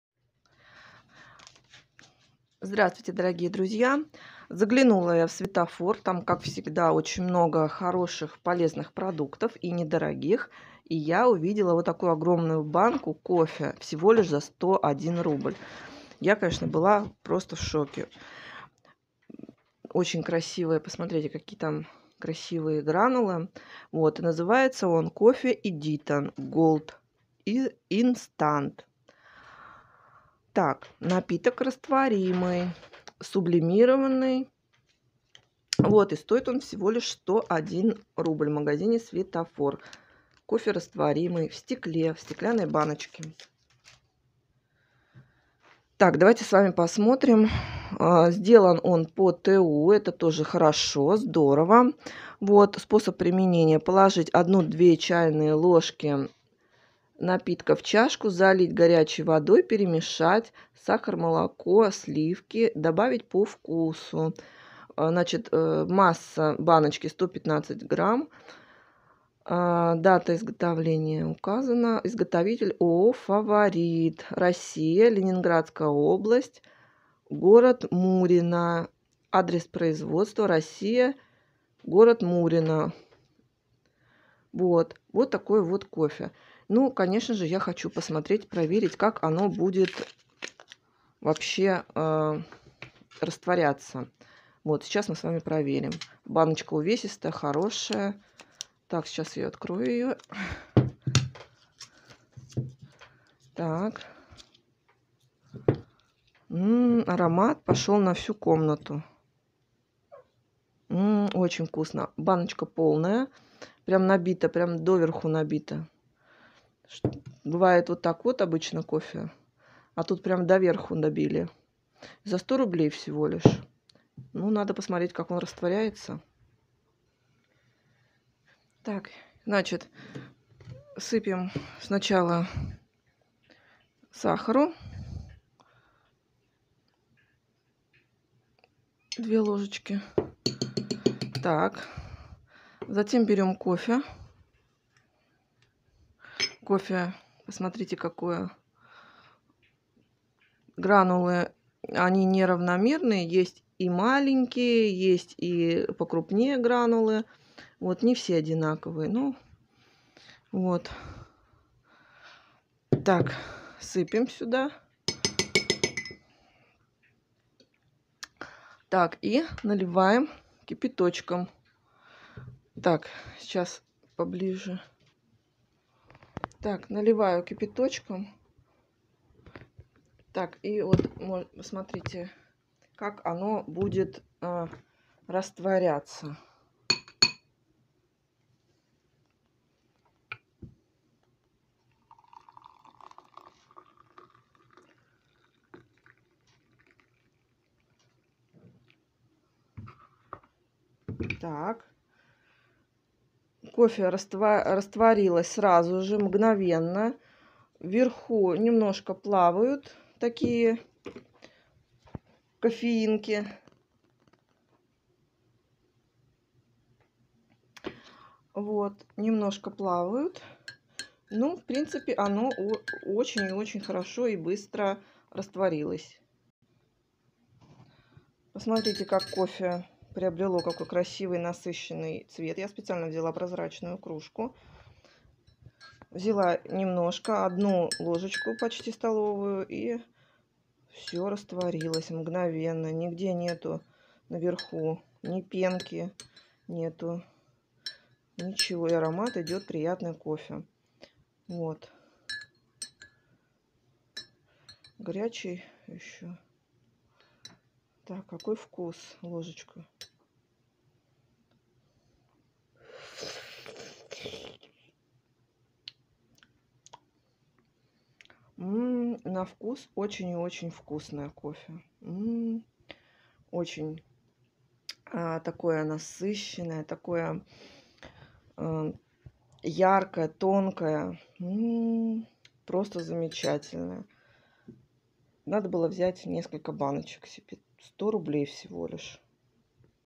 НОВИНКА🚥СВЕТОФОР🚥ОБЗОР/НОВИНКА КОФЕ Edition Gold ЗА 100 РУБЛЕЙ
Купила в Светофоре новинку кофе Edition Gold всего за 100 рублей, налила в чашку, пробую на вкус.